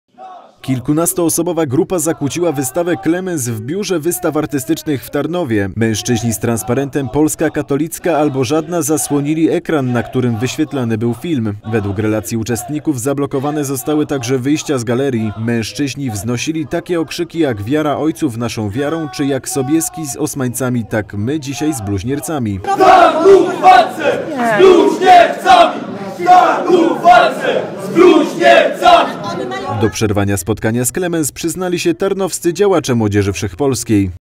Młodzież Wszechpolska przerwała spotkanie z Kle Mens! - Magazyn Miejski 06/04/2016 - imav.tv
Kilkunastoosobowa grupa zakłóciła wystawę Kle Mens w Biurze Wystaw Artystycznych w Tarnowie. Mężczyźni z transparentem "Polska katolicka albo żadna" zasłonili ekran, na którym wyświetlany był film. Wg relacji uczestników zablokowane zostały także wyjścia z galerii. Wznosili okrzyki np. „Wiara ojców naszą wiarą” oraz „Jak Sobieski z osmańcami, tak my dzisiaj z bluźniercami”.